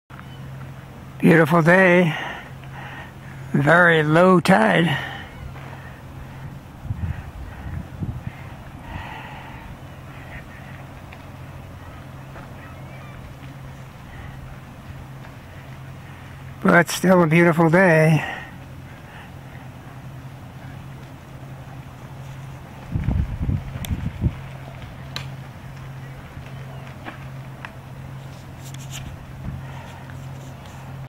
Low down tide view of Carquinez Strait as seen from vista point at the BSRA.
Low down tide view of Carquinez Strait as seen from vista point at the BSRA with Mount Diablo in the background.